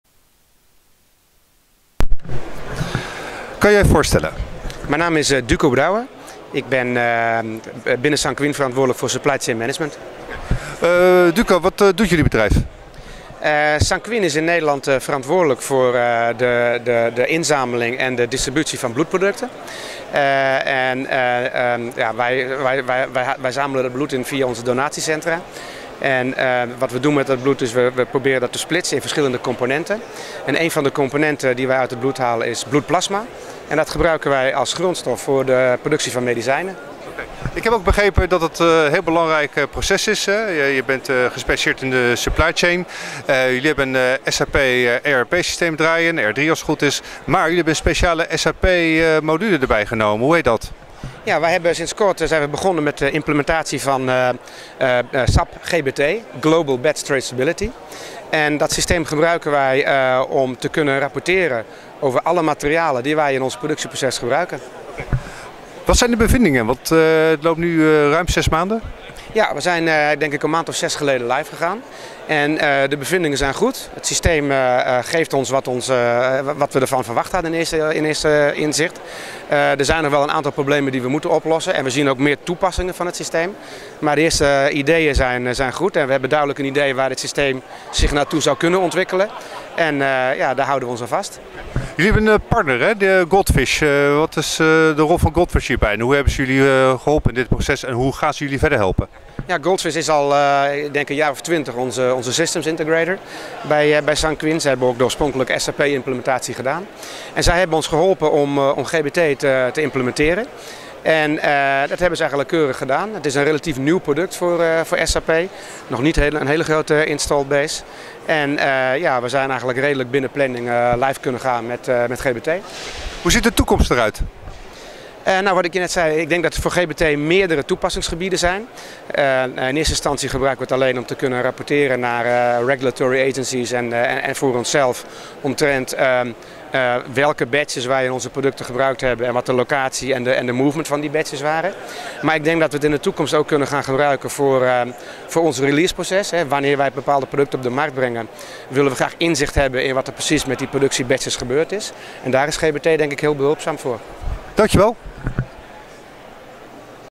SAP SAPPHIRE NOW update met Duco Brouwer SANQUIN PLASMA PRODUCTS
SAP SAPPHIRE NOW 2017 update met Duco Brouwer van SANQUIN PLASMA PRODUCTS

Millions of data sets being analyzed and “protected” so has a nice compelling story* Innovation Case: Sanquin; Saving lives of people that need blood or plasma derived medication fast; Millions of people donate plasma, giving blood plasma enables them to contribute to patients that have rare and life threatening diseases or injuries. Plasma is derived from blood or can be directly donated (plasmaphereses). The plasma donation is then labeled, sampled, frozen and managed in a software application. These applications contain privacy sensitive information, but are vital in cases where a donor poses risk due to explicit behavior or external factors like tropical disease or tattoo’s. Although current screening and pathogen killing processes mitigate risks for contaminated plasma, there can be reasons for plasma to be recalled fast and adequate. As millions of donations pass through the supply the chain and are being recorded in several IT-systems across companies and legal boundaries. You can imagine, traceability can become a very complex, time consuming and error prone process. Now, new insights and possibilities can boost performance, reduce risk and time to report. Integrating all non-privacy related data, in a single systems and, visually represent the supply chain from donor to packed product can be done with the tools and software of today. SAP Global Batch Traceability, is a system that is designed for fast reporting over vast amounts of data across systems.